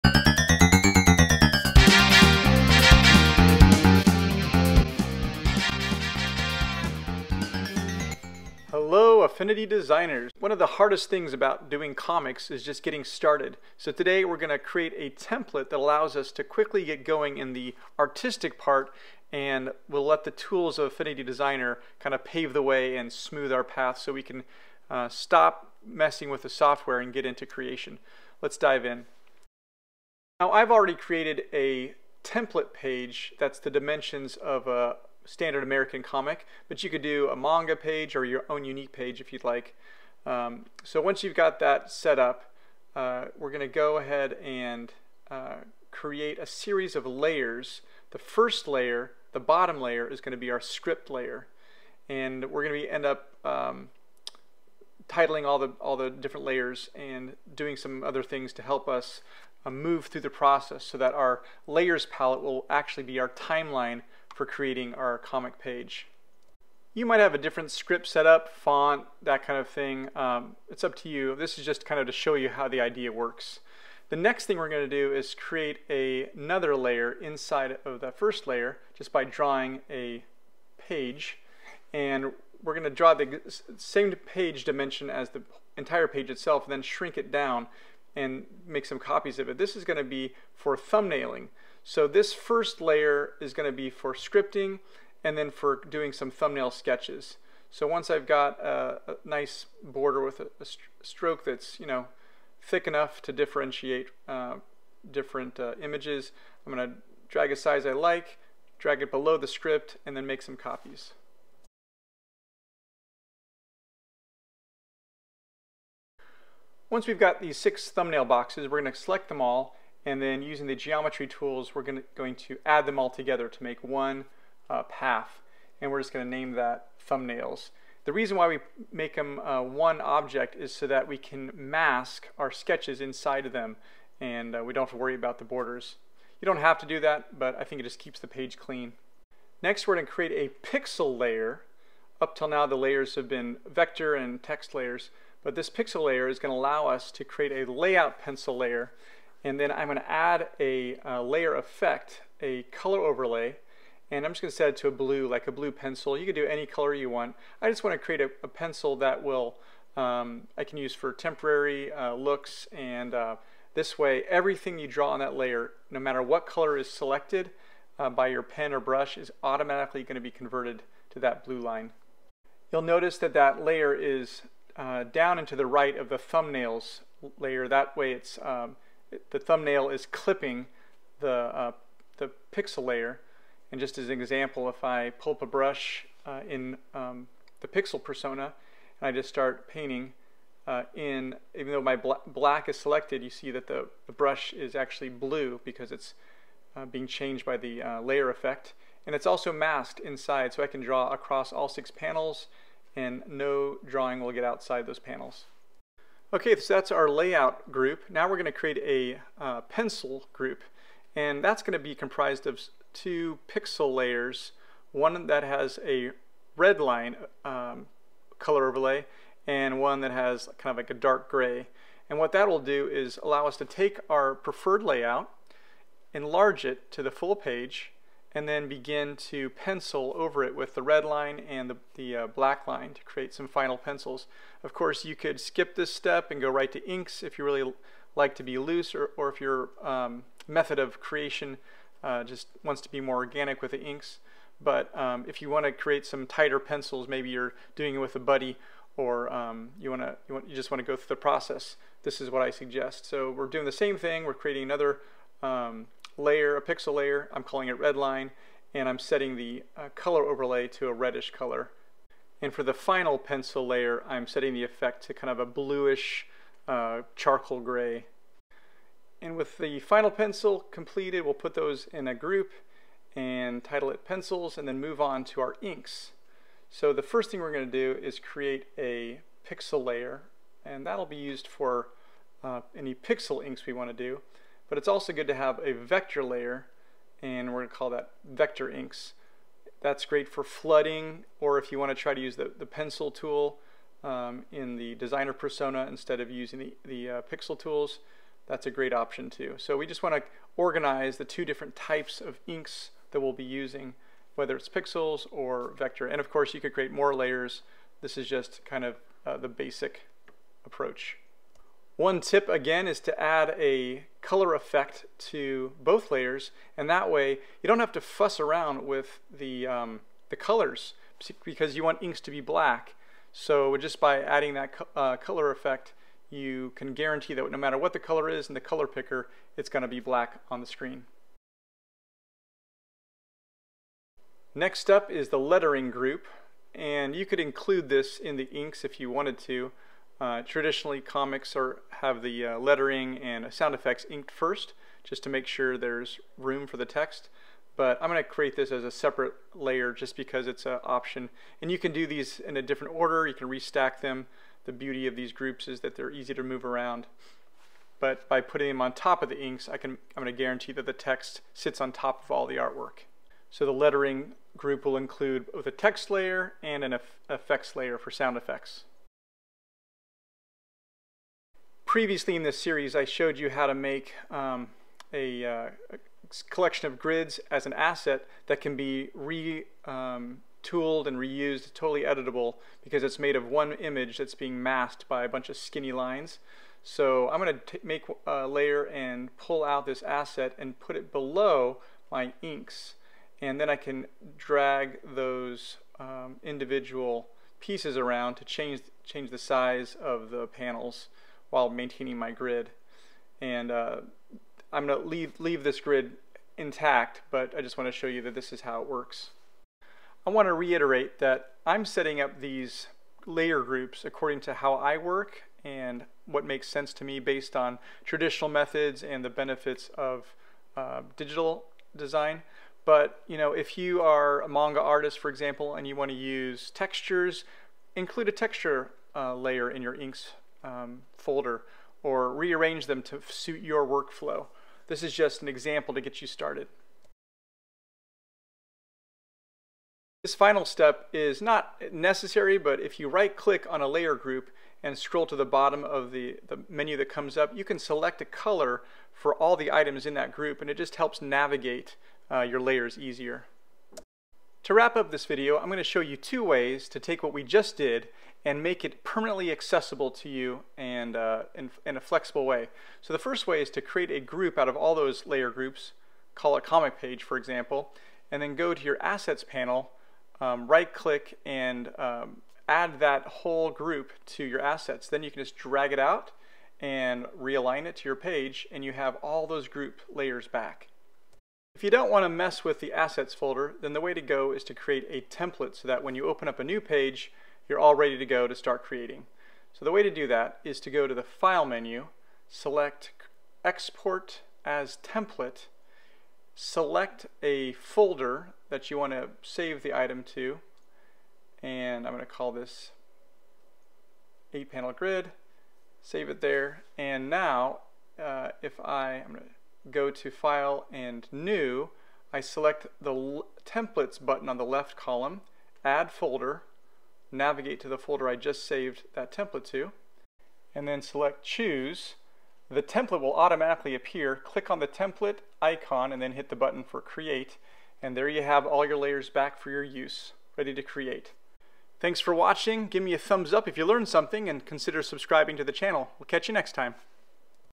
Making Comics w/ Affinity Designer: Pt. 7 Ready Your Comic Space Pt.1
One of the hardest things to do when making comics is just getting started. This two-part tutorial aims to let Designer remove the obstacles to creating comics by creating a reusable set of layers for each step of the process.

To learn how to create reusable panels in the Assets studio, watch this:

0:00 Intro
0:09 Overview
0:51 Script/Layout Group
3:26 Pencil Group
4:30 Ink Group
5:34Text & FX Group
7:20 Color Coded Layers
7:34 Creating and Asset
8:01 Creating a Template